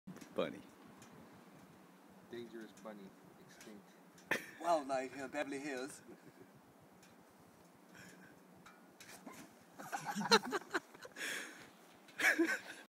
Bunny hunting
Tuan's attempt to catch the wild neighborhood bunny